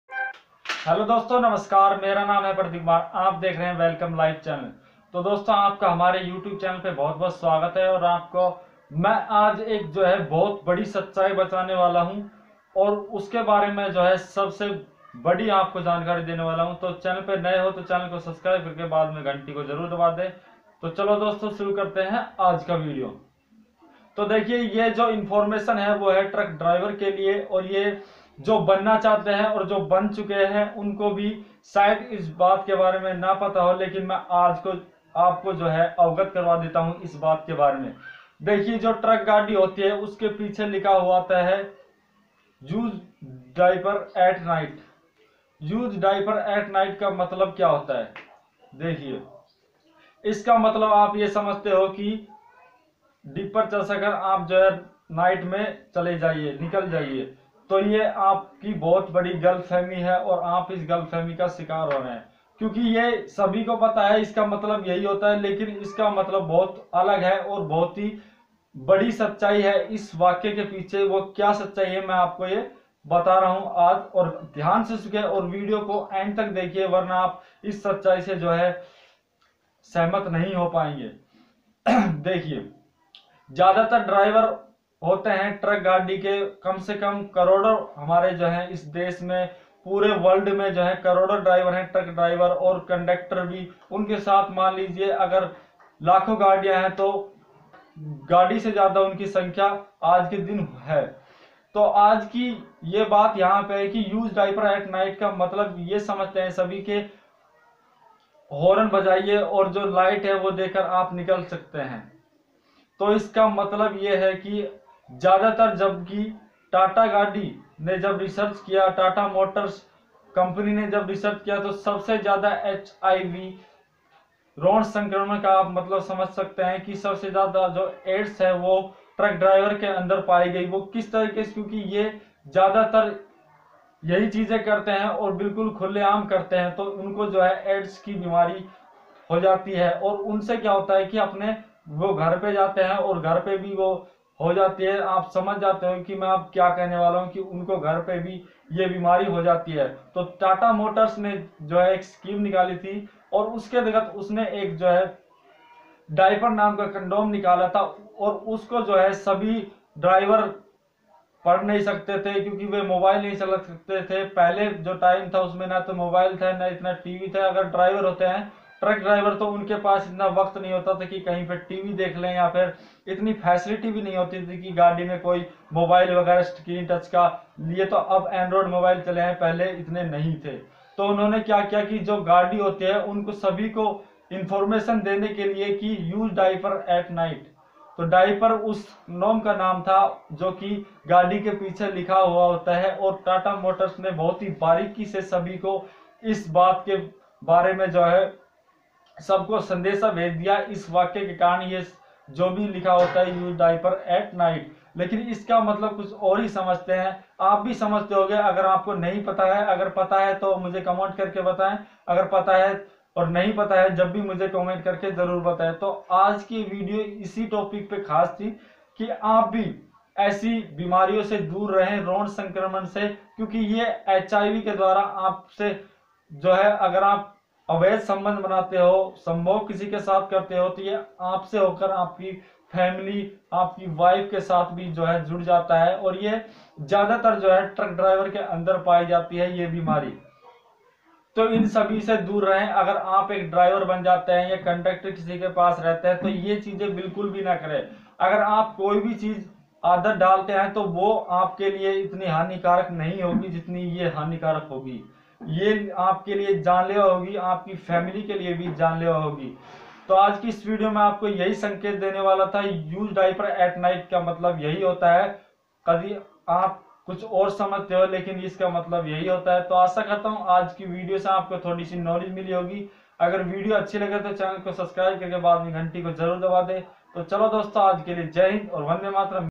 Truck Ke Piche use Dipper at night ka matlab Jaane Sachai
is video mein main aapko use dipper at night me  baare mein Batayehein